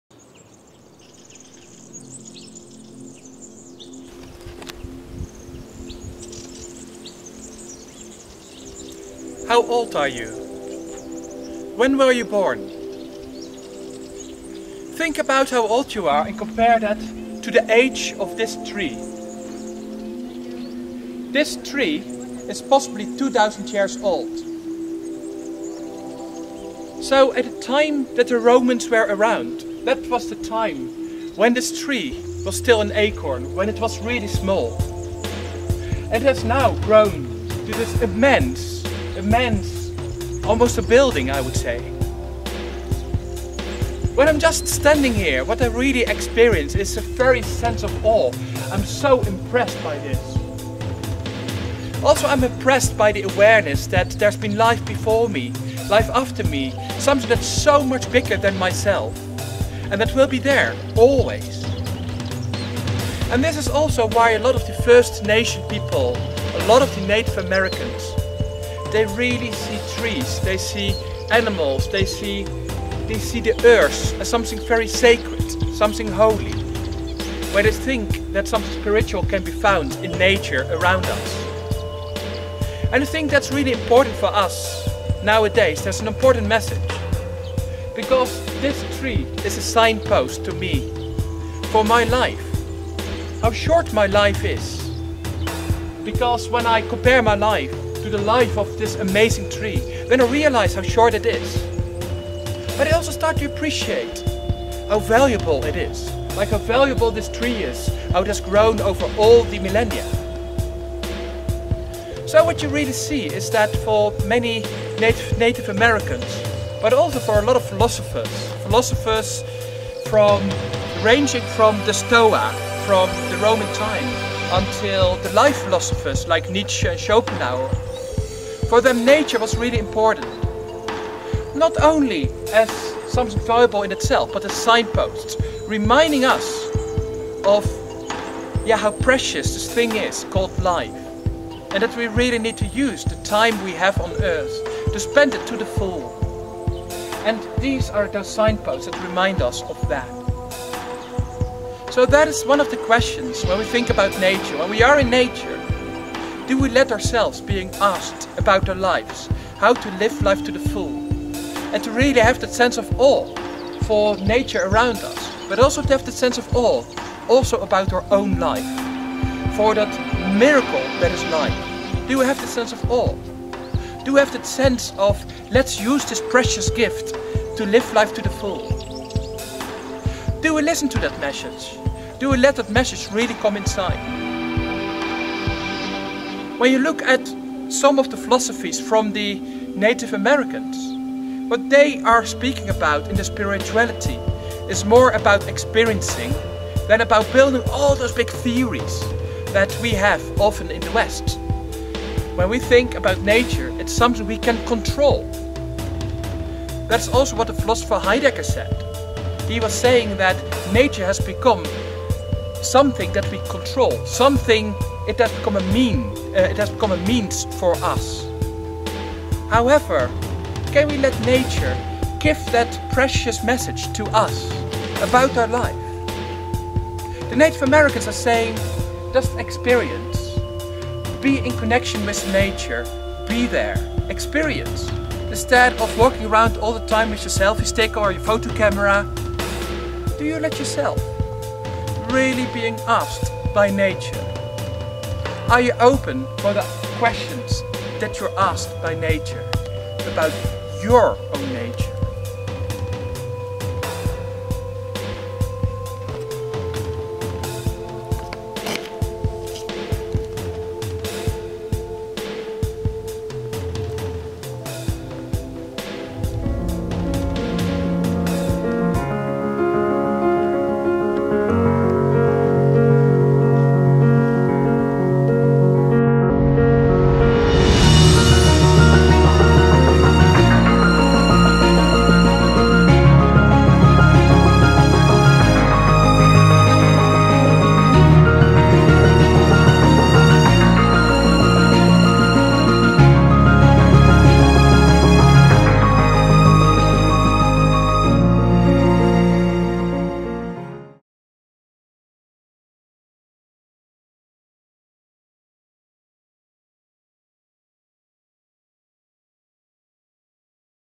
The age of the sequoias: how to accept your age and develop a larger perspective on life
When we compare our life span with the age of ancient trees, we can become aware of how relatively small we are. We can connect with nature to develop a larger perspective on ourselves.
This video is made in Yosemite National Park, USA.